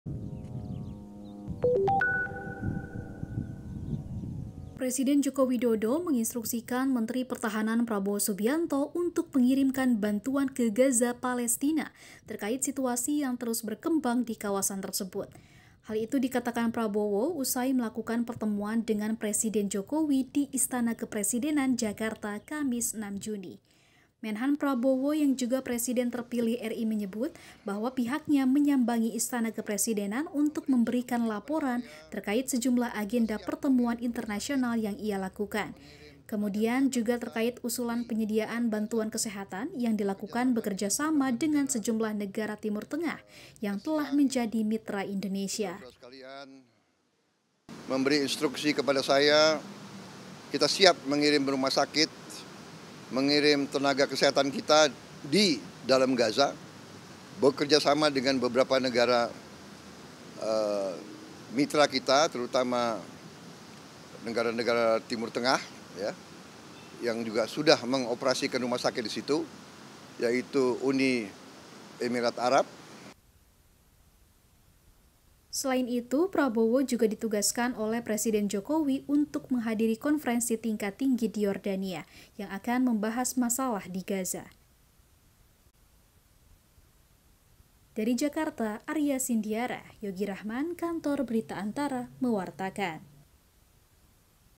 Presiden instruksikan Menhan kirim bantuan rumah sakit ke Gaza
ANTARA - Menteri Pertahanan Prabowo Subianto menyebut bahwa Presiden Joko Widodo memberikan instruksi untuk mengirimkan bantuan tenaga kesehatan hingga penyediaan rumah sakit lapangan ke Gaza, Palestina. Hal itu dikatakan Prabowo usai bertemu dengan Presiden di Istana Kepresidenan Jakarta, Kamis (6/6). (Aria Cindyara/Yogi Rachman/Rizky Bagus Dhermawan/Gracia Simanjuntak)